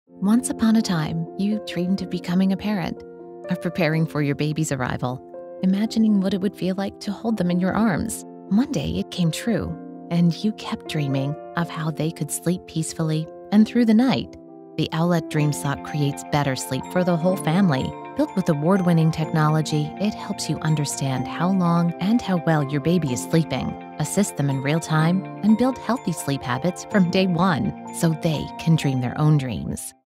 Meet the New Owlet Dream Sock
The NEW Owlet Dream Sock is here. Build healthy sleep habits from day one with sleep tracking, sleep assist prompts, and unlimited access to our award-winning digital sleep coaching program.

*US only